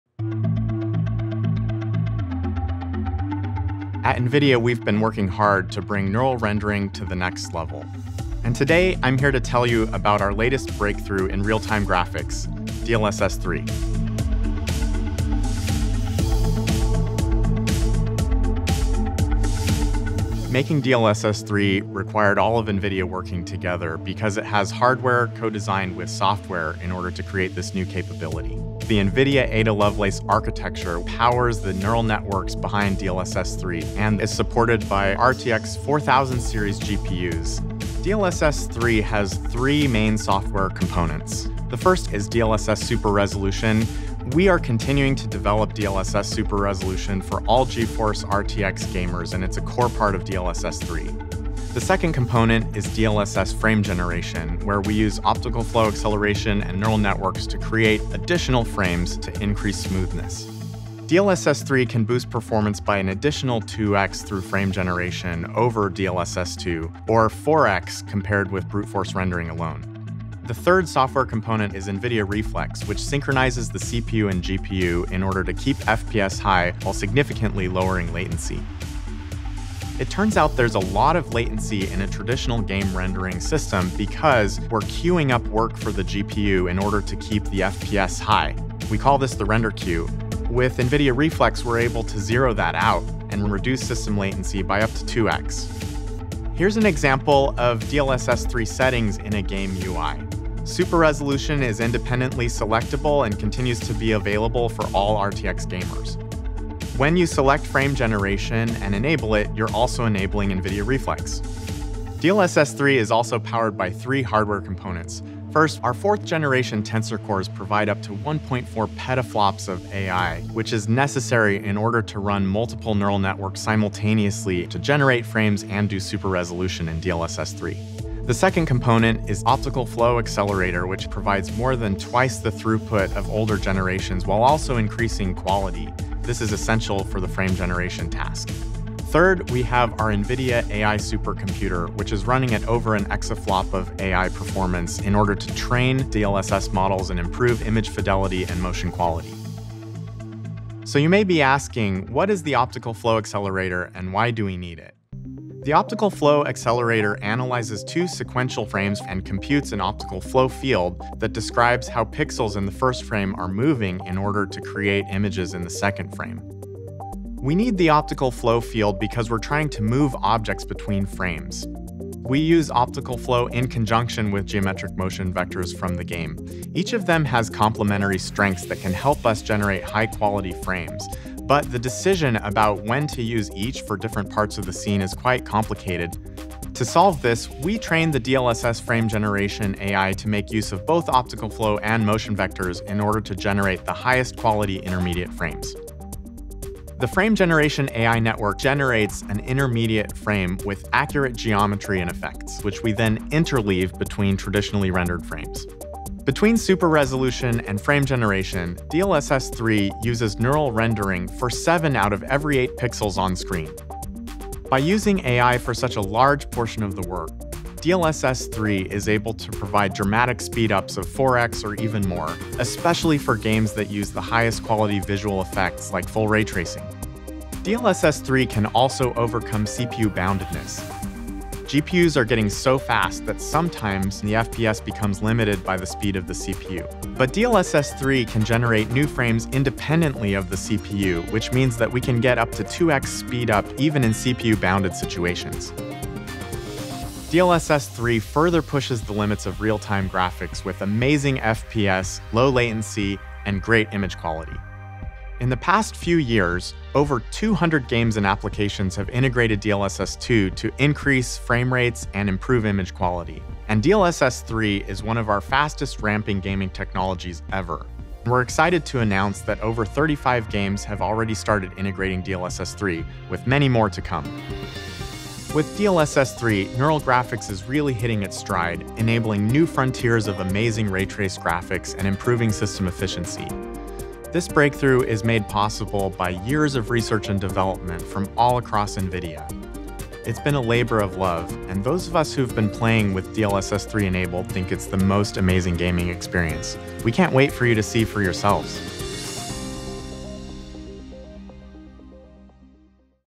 NVIDIA DLSS 3 とは何か？AI で性能が飛躍する (日本語字幕付き)
※字幕をご覧になるには、設定で日本語字幕を有効にしてください。

NVIDIA Applied Deep Learning Research チームの Bryan Catanzaro が、ゲームやアプリの性能を飛躍させる革新的な AI グラフィックス技術である NVIDIA DLSS 3 について解説します。DLSS 3 を構成する 3 つの技術である オプティカル マルチ フレーム生成、超解像度、NVIDIA Reflex について詳しく説明します。

DLSS 3 は DLSS 2 と後方互換性があり、35 以上のゲームやアプリがすでにこの技術への対応を開始しており、最初の対応タイトルは 2022 年 10 月より順次配信されます。

「NVIDIA GeForce JP」YouTube チャンネルに登録しましょう !

「NVIDIA GeForce」日本公式ソーシャルアカウントから最新情報をお届け！

究極の PC ゲーミング 体験を実現する「NVIDIA GeForce」の最新情報はこちら